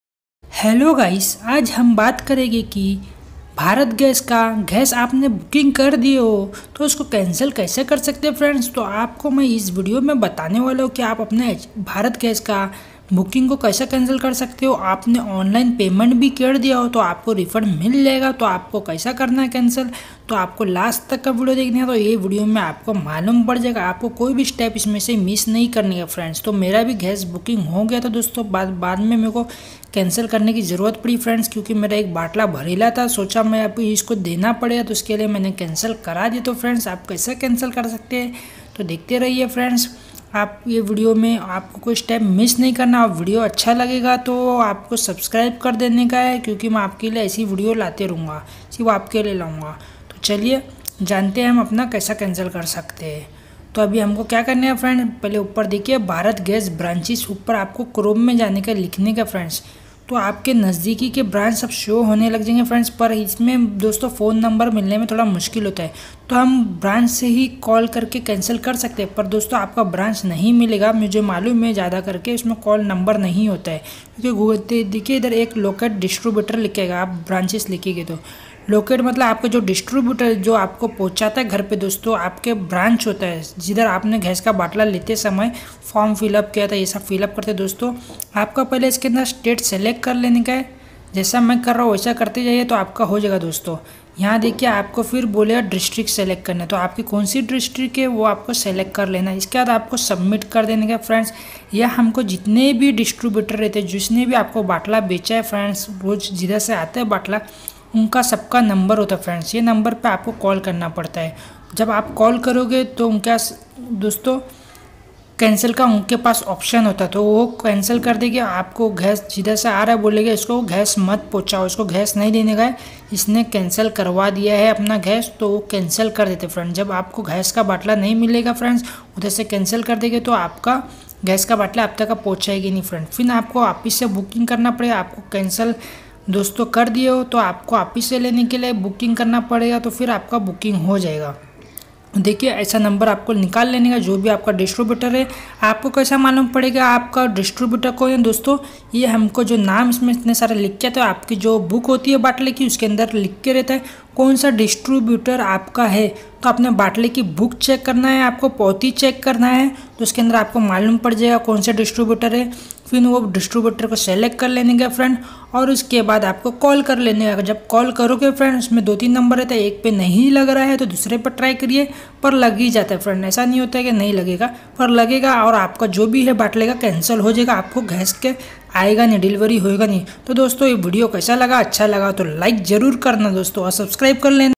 bharat gas booking cancel kaise kare | how to cancel Bharat gas booking
in this video i am talk about Bharat gas booking cancel kaise kare.